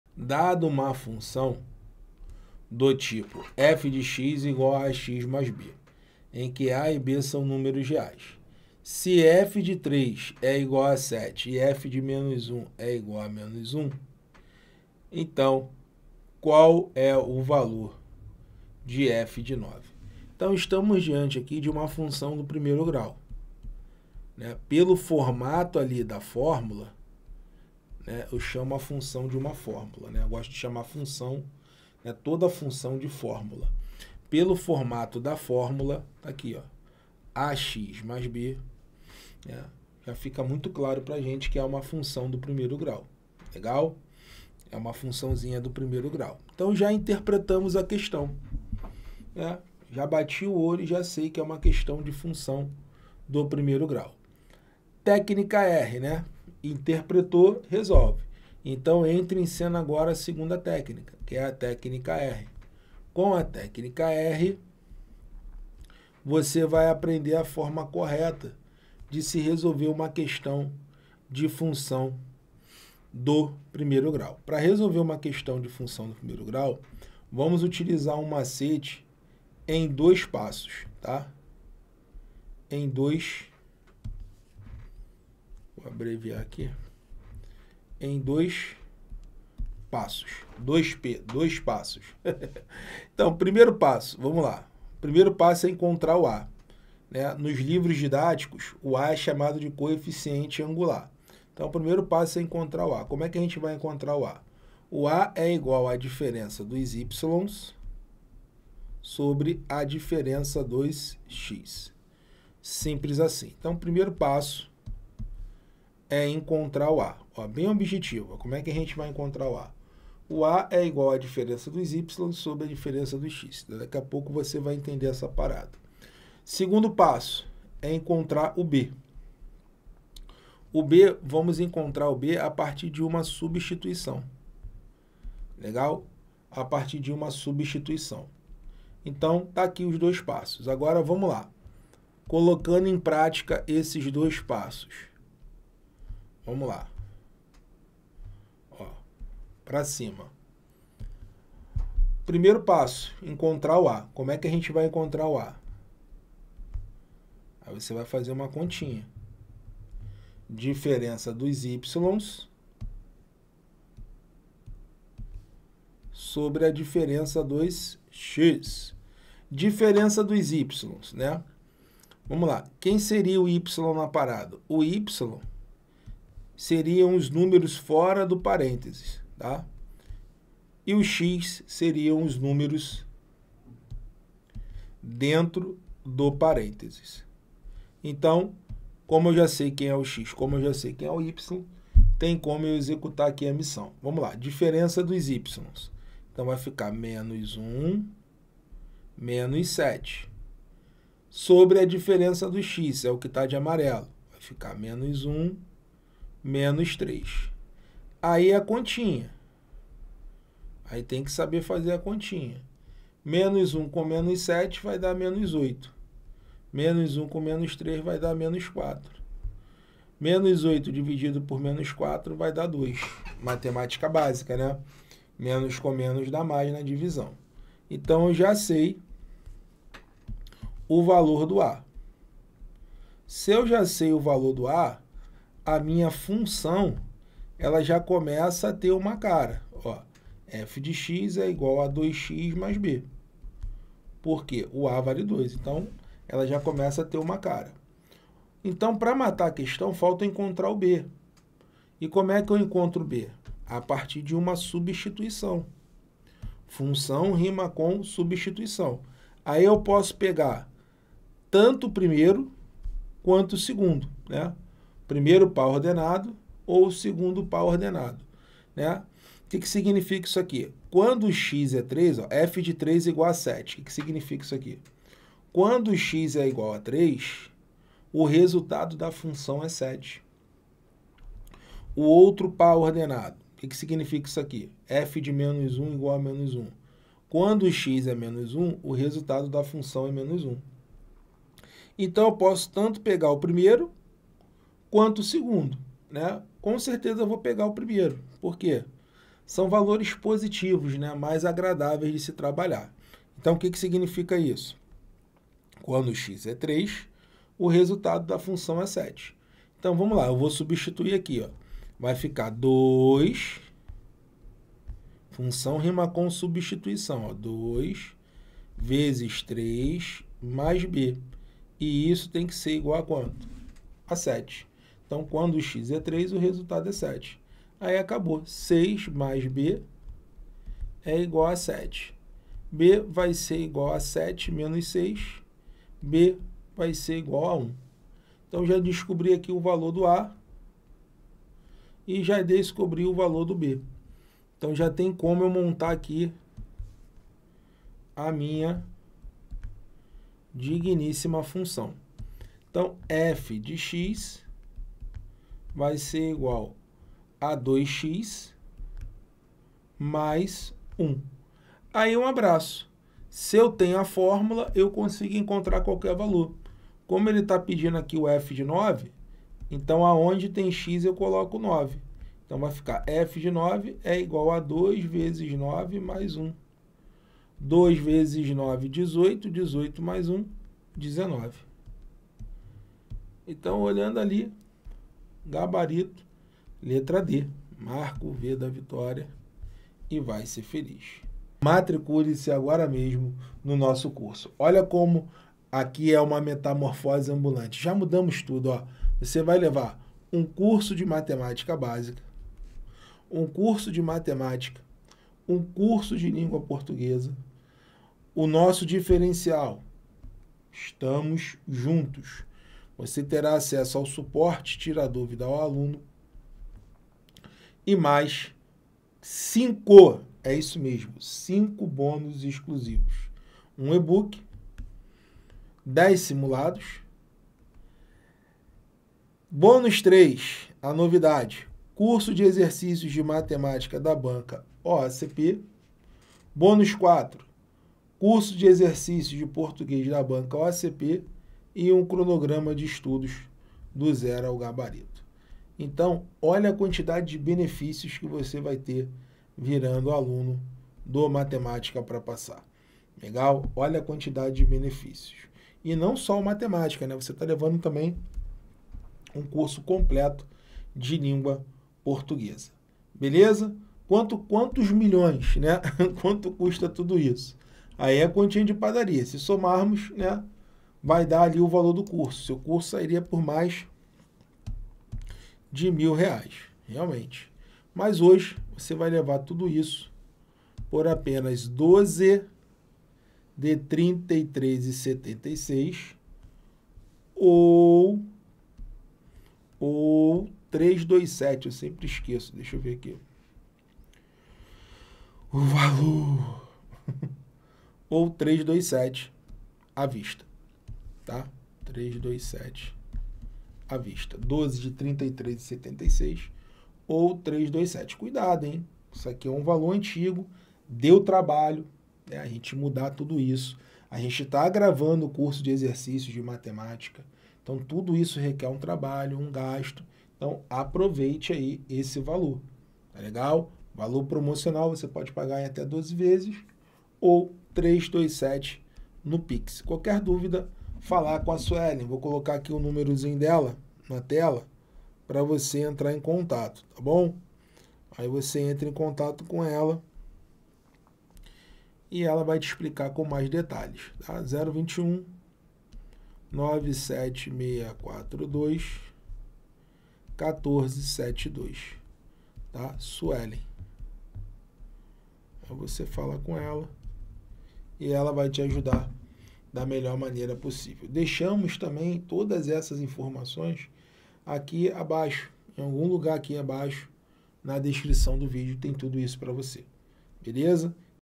Matemática para PM PE | Função como você nunca viu
Então você precisa aprender com esse vídeo onde nosso professor, Marcão, mostra Matemática para PM PE. Para garantir muitos pontos na prova da Matemática e mandar muito bem na prova da PM PE

Para você ser aprovado na PM PE precisa mandar muito bem em matemática e com esse vídeo você irá ter um Guia Prático para interpretar e saber o que utilizar nas questões de Matemática da Banca AOCP.